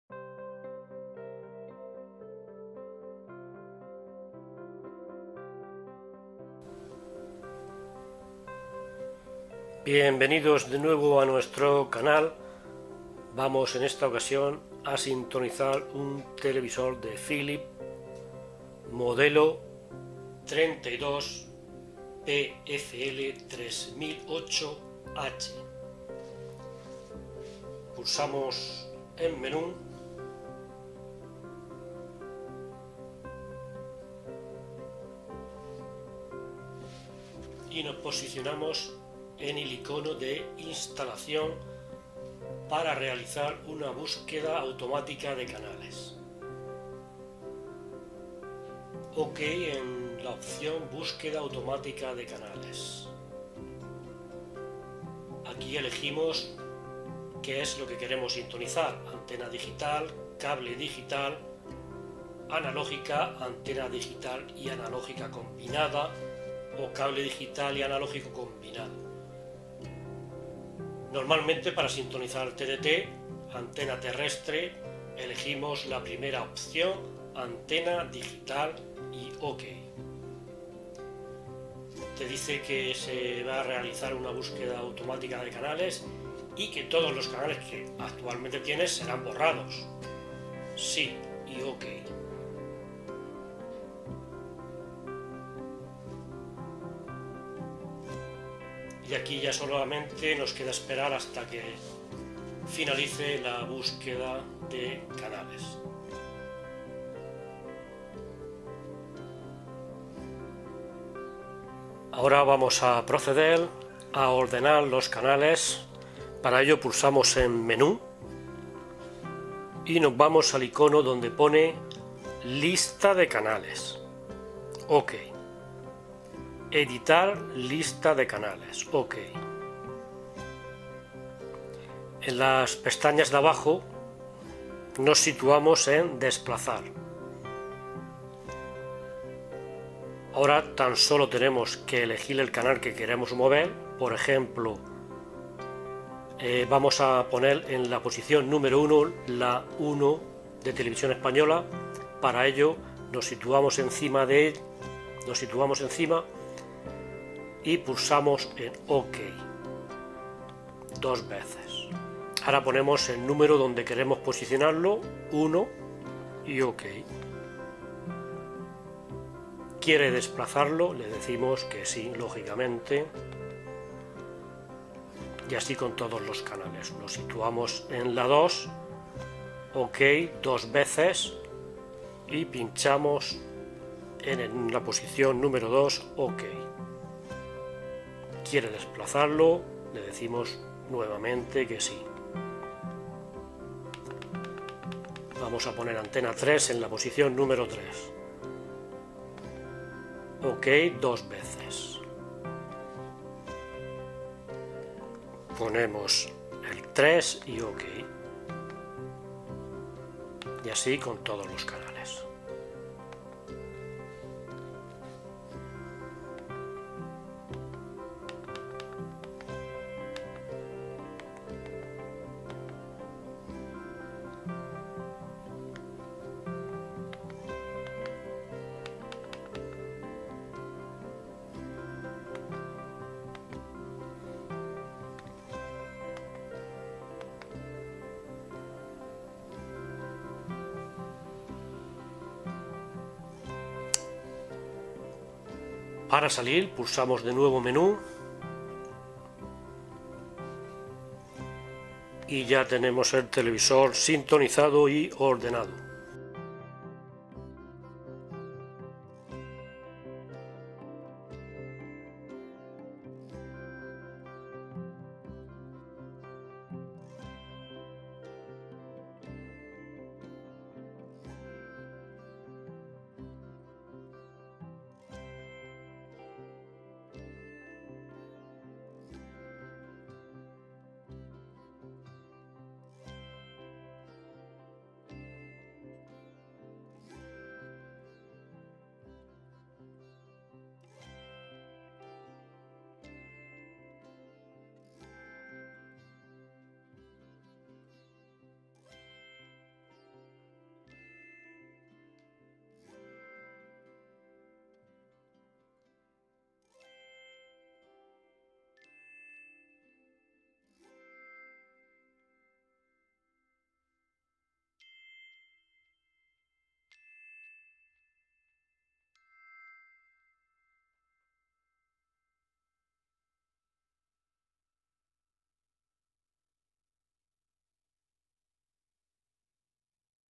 Buscar y ordenar canales tv Philips 32PFL3008 🥇
🥇 Te mostramos cómo sintonizar y ordenar canales en TV Philips 32PFL3008 y similares de la misma serie. Este televisor está fabricado por VESTEL y difiere de modelos recientes que son de fabricación CHINA. Es para el modelo mencionado y otros de la misma serie.